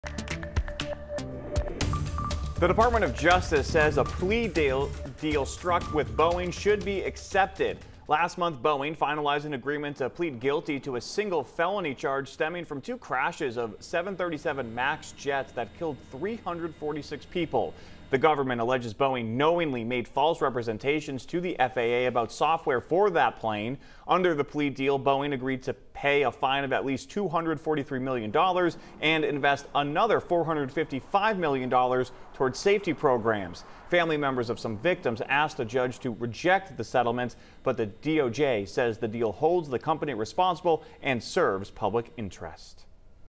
Department of Justice defends Boeing plea deal
Boeing agreed to pay a fine of at least $243 million.